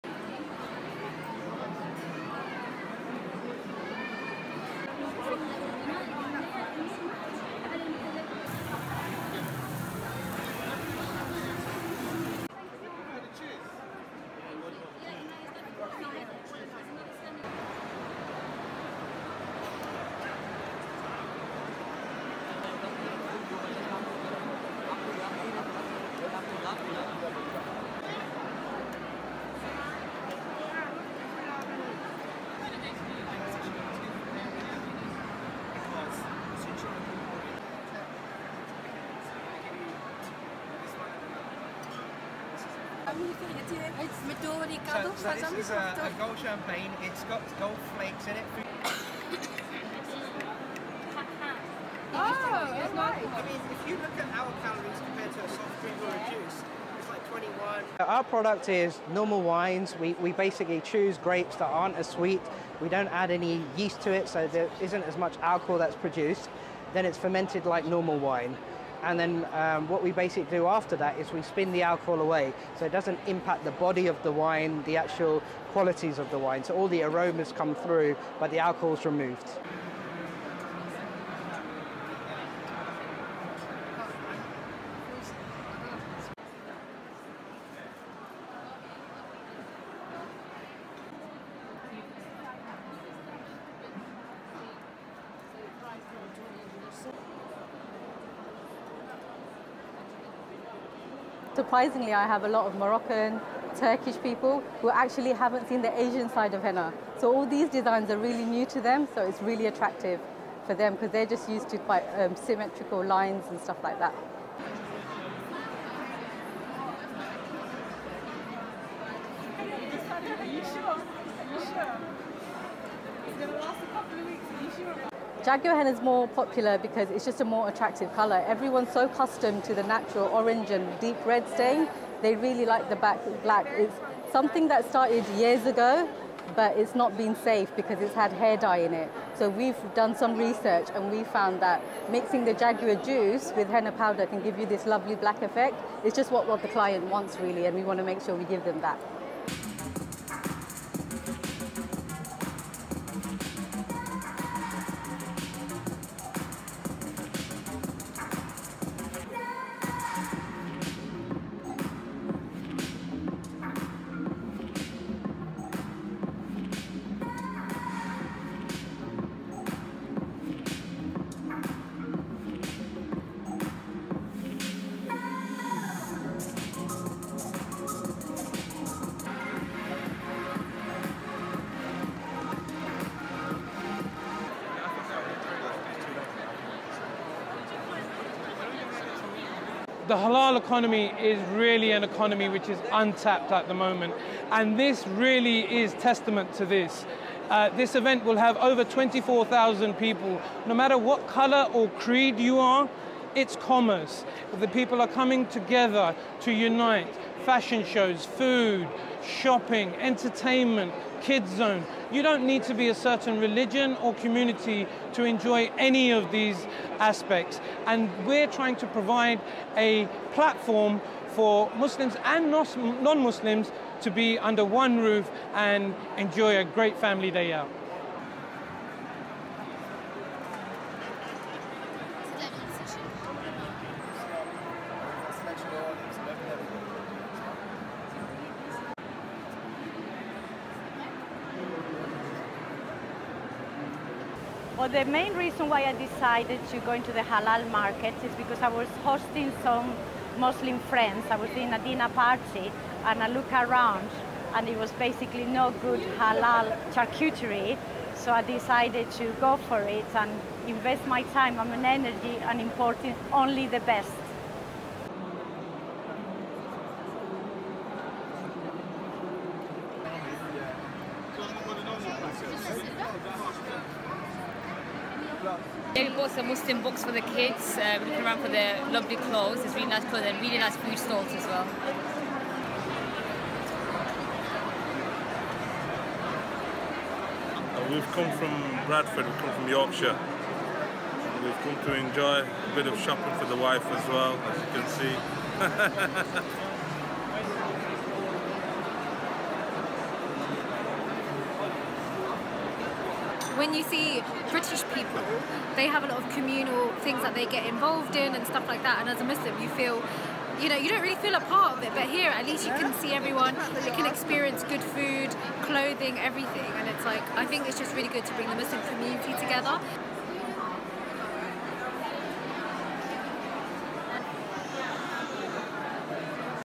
From modest fashion to non alcoholic wine and halal delights
(23 Apr 2018) LEADIN:
From modest fashion to non alcoholic wine and halal charcuterie, the London Muslim Lifestyle Show claims to offer something for everyone.
More than two hundred Muslim-focused businesses set up shop in London for the event, and organisers say they're aiming to appeal to non-Muslims too.
STORYLINE
Whether it's Islamic inspired art or lighting for the home, a tasty halal snack, or the latest in Muslim women's fashion, the London Muslim Lifestyle Show caters for all kinds of tastes and desires.
The show is billed as a one-stop-shop for Muslim fashion, food and live entertainment, and organisers are expecting about 20,000 visitors from across the UK and overseas over two days.
One thing you might not expect to see here is a wine merchant.
Muslims across the world traditionally shun alcohol, but one company claims to have created a non-alcoholic wine that actually tastes like the real thing.
Azhar Paul runs Azanti, which sells a variety of non alcoholic wines, including a sparkling wine containing flakes of gold.
He says the alcohol produced during fermentation is removed later in the process, leaving behind the taste of the wine.
"Our product is normal wines. We basically choose grapes that aren't as sweet. We don't add any yeast to it so there isn't as much alcohol that's produced, then it's fermented like normal wine. And then what we basically do after that is we spin the alcohol away so it doesn't impact the body of the wine, the actual qualities of the wine, so all the aromas come through but the alcohol is removed," he says.
Something more traditional on offer is henna painting.
It's used throughout the Middle East and Asia as a means of adorning the hands, arms and feet and is traditionally done during times of celebration.
Here at Red Henna, artists are applying intricate south Asian patterns onto visitors' hands.
"Surprisingly I have a lot of Moroccan, Turkish people who haven't actually seen the Asian side of henna. So all these designs are really new to them so it's really attractive for them because they're just used to quite symmetrical lines and stuff like that," says company owner Razia Sultan.
Decorative henna is normally a reddish colour when applied, but Razia uses a special blend of henna powder and extract of jaguar fruit which produces the colour black.
"Jagua henna is more popular because it's just a more attractive colour. Everyone is so accustomed to the natural orange and deep red stain, they really like the black. It's something that started years ago but it's not been safe because it's had hair dye in it. So we've done some research and we've found that mixing the jaguar juice with henna powder can give you this lovely black effect. It's just what the client wants really and we want to make sure we give them that," she says.
One of the most keenly anticipated events here is the Modest Fashion Show.
Models show off the latest trends in loose fitting garments, headscarves and abayas - like these by fashion label Meraki Melon.
Organiser Waleed Jahangir says he hopes the event will have plenty to appeal to non-Muslims too, whether its fashion, food or entertainment.
Now in its third year, the event has grown from a hundred exhibitors in the first year, to 250 exhibitors from 15 countries this year.
Several food stalls are tempting visitors with halal versions of foods that are typically not halal, such as cured meat.
Encina Barragan, owner of The Ojos Foods, says she realised there was a gap in the market.
Mehreen Paraouty says it's good to have an event that showcases a culture she feels she can identify with.